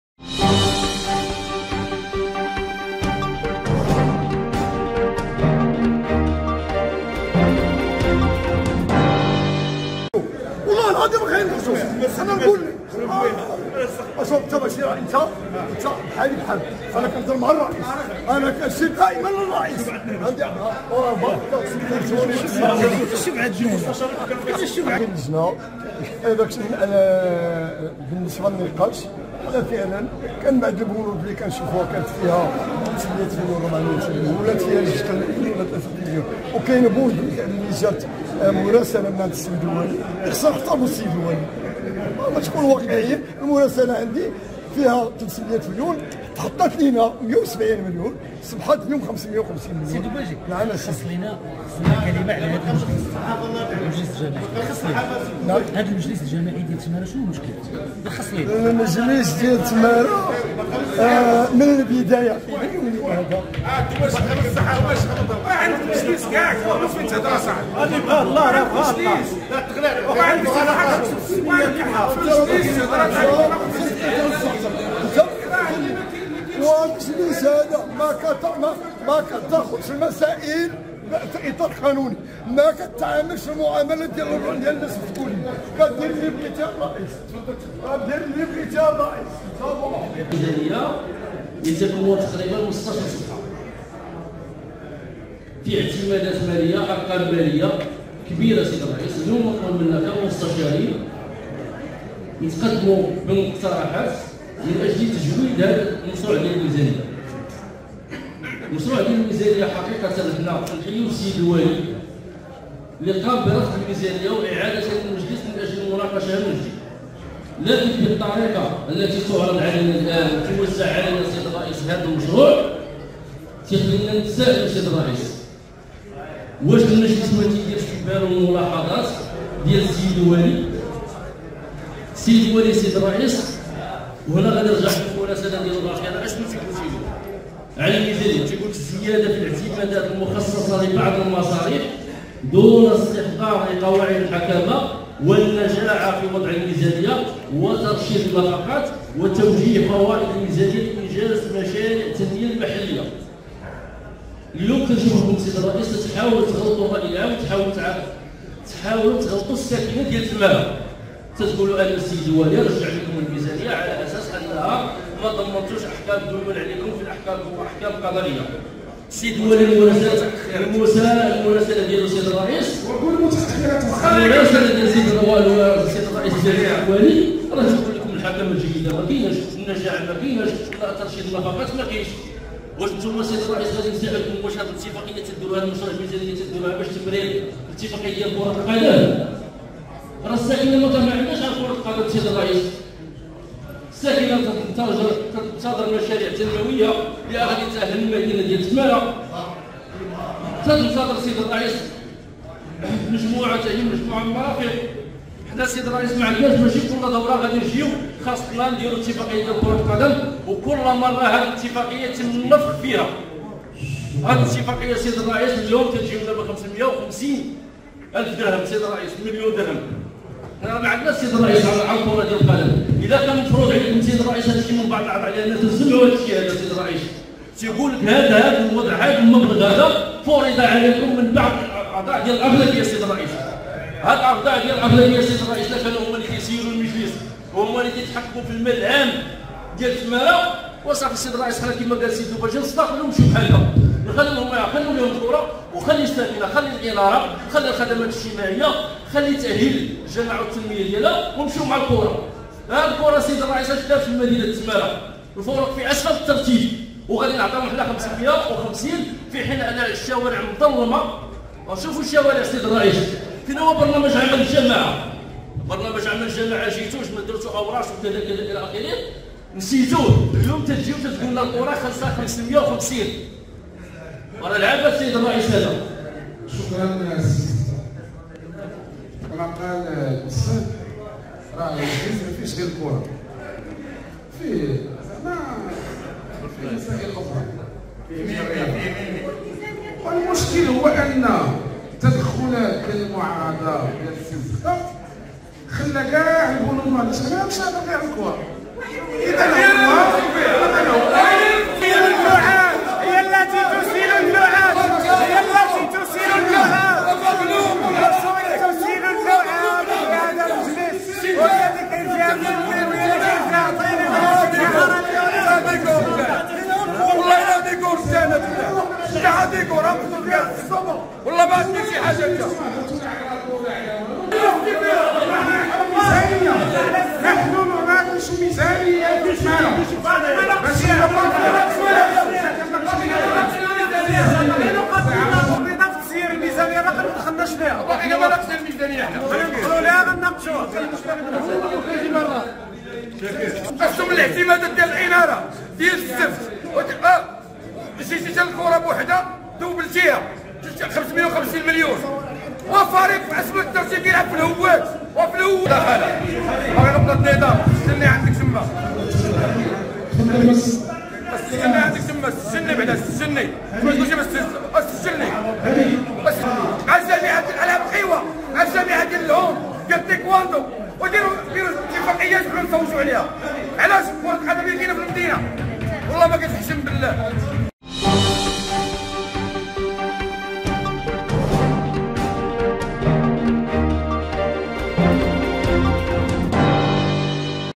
550 مليون سنتيم منحة الفرق الرياضية بجماعة تمارة تسيل اللعاب على حد تعبير مستشار من المعارضة
550 مليون سنتيم  - منحة الفرق الرياضية  - بجماعة تمارة  - تسيل اللعاب -  على حد تعبير  - مستشار من المعارضة -